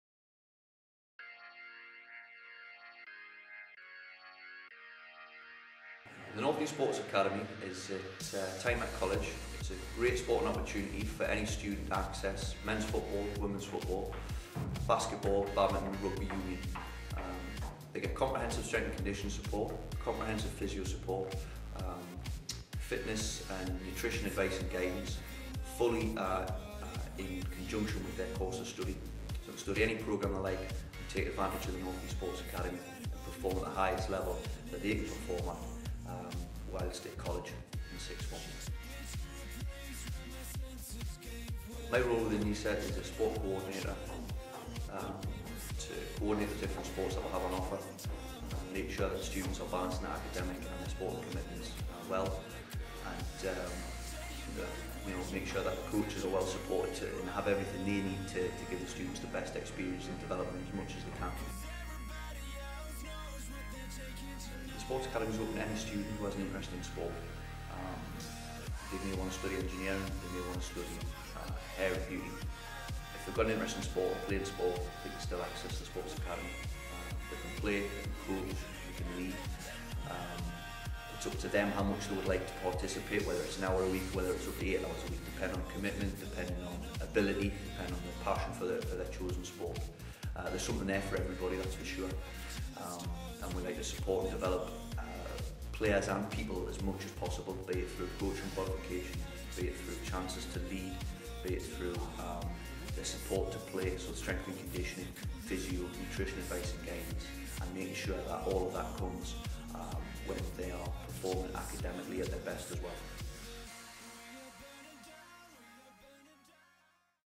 What is The North East Sports Academy?
NESA Sport Coordinator Mark McCutcheon explains more about the North East Sports Academy.